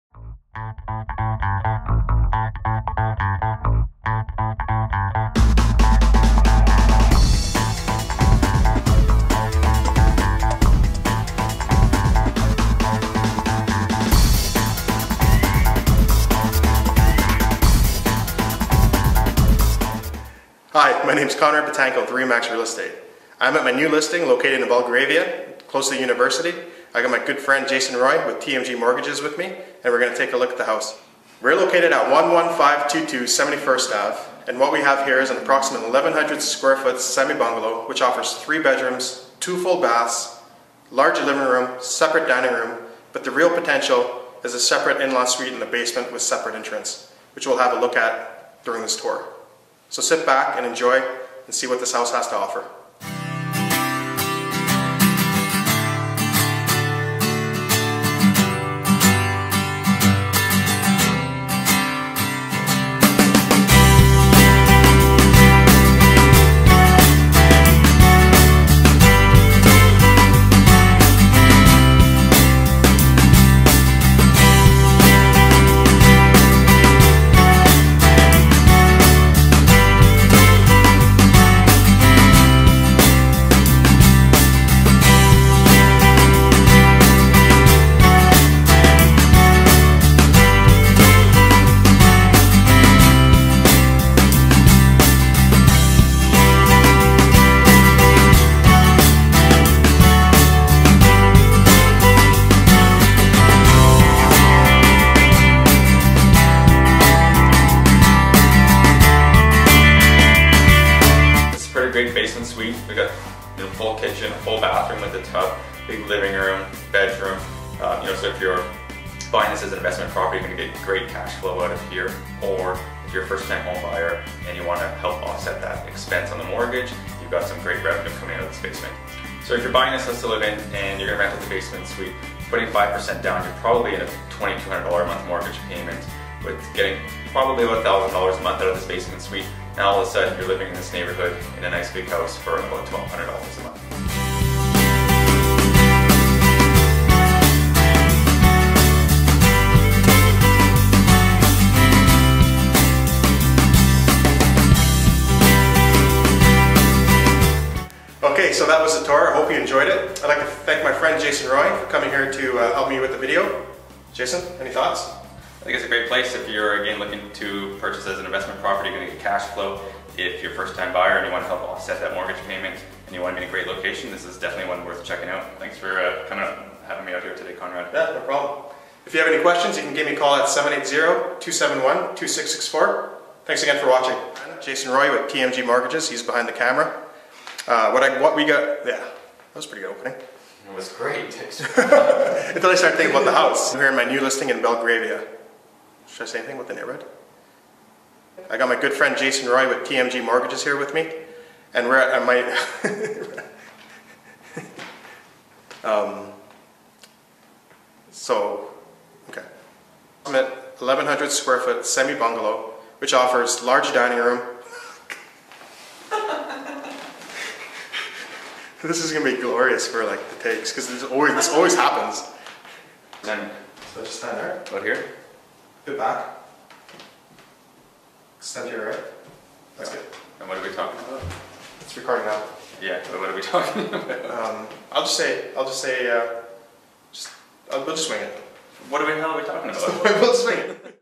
Edmonton Home for Sale | 11522 71 Ave (Belgravia) | Presented by Conrad Bitangcol
Property Details:

Revenue earning potential in Belgravia!!  This solid 1023 sq.ft. semi-bungalow offers 4 bedrooms & 2 full bathrooms.  The main floor features a large separate dining room which is open to the bright living room, kitchen with a small breakfast nook, full bathroom & a spacious bedroom with 2 closets.  The main floor windows have been replaced.  Upstairs there are 2 additional bedrooms & lots of neat areas for storage.  The basement features an in-law suite with a separate entrance & offers excellent revenue potential.  There's a large kitchen area, big bright living room, full 4 pce bathroom & a large bedroom. Steps to the UofA, Whyte Avenue, the River Valley, LRT & local schools.  The large lot is full of mature trees & is south facing so it keeps the living areas inside sunny throughout winter.  This is an excellent opportunity in a prime location & will go fast!! Don't miss out!!

Conrad Bitangcol
RE/MAX Real Estate